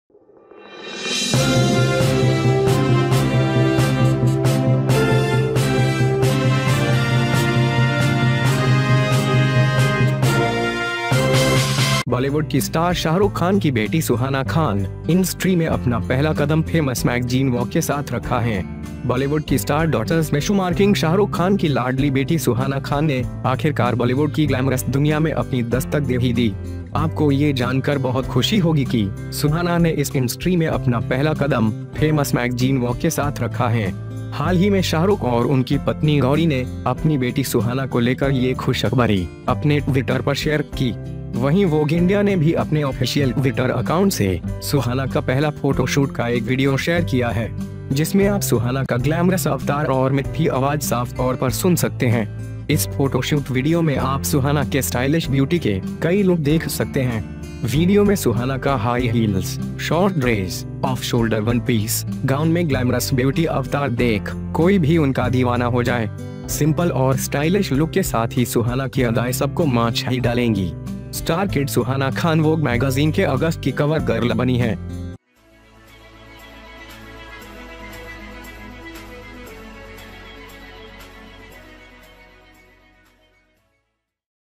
शाहरुख की बेटी सुहाना ने हिला डाला सबको, देखके आप भी हैरान रह जायेंगे...
Suhana Khan, daughter of Bollywood star Shah Rukh Khan, has put her first step in the industry with Famous Magazine Vogue.
Shah Rukh Khan's ladylike daughter, Suhana Khan, starring Bollywood's Star Doters, finally gave her a knock in the glamorous world of Bollywood. You will be happy to know that Suhana has kept his first step in this industry with Famous Magazine Vogue.
Recently, Shahrukh and his wife, Gauri shared this good news about their daughter Suhana, on her Twitter. Vogue India also shared a video of Suhna's first photoshoot with her official Twitter account. In which you can hear Suhana's glamorous avatar and hilarious voices clearly. In this photoshoot video you can see many of the stylish beauty of Suhana. In the video, Suhana's high heels, short dress, off-shoulder winpiece, look at the glamorous beauty of the gown and anyone can get her crazy. With a simple and stylish look, Suhana's paycheck will kill everyone. Star Kid Suhana Khan became the cover girl of Vogue magazine in August.

HJ NEWS !!